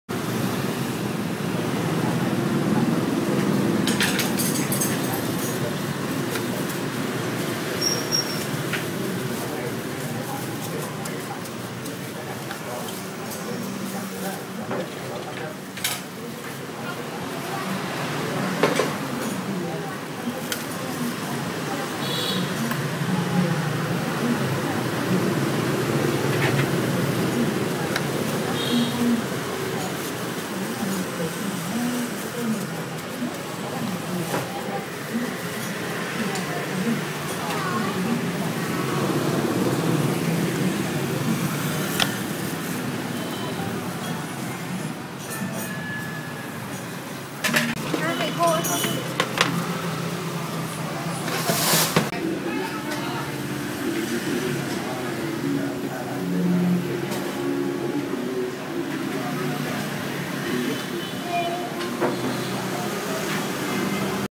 VietNam Foods - Stuffed Pancake - Gone Cake - Bánh Cuốn Việt Nam
Gone cake
  Goods coated Gone cake   this around the neighborhood and because the medium is Snacks Breakfast delicious, easy to eat healthy
Bánh Cuốn
 Hàng tráng bánh Cuốn như thế này có ở khắp các khu dân cư ,vưa là Quà Vặt vừa là Bữa Sáng ngon , lành dễ ăn